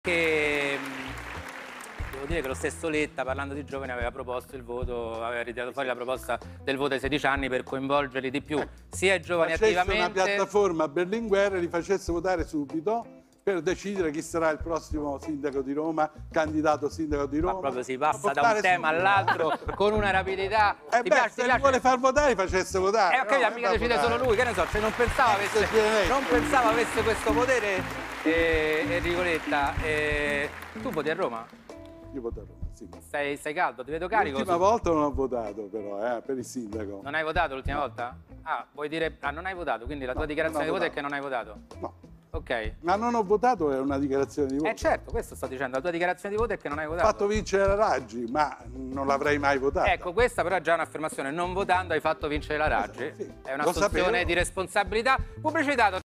Michele Santoro a Enrico Letta: "Fai una piattaforma Berlinguer per il voto ai 16enni"
Voto ai 16enni, Michele Santoro: "Letta facesse una piattaforma Berlinguer e li facesse votare subito per decidere chi sarà il prossimo sindaco di Roma".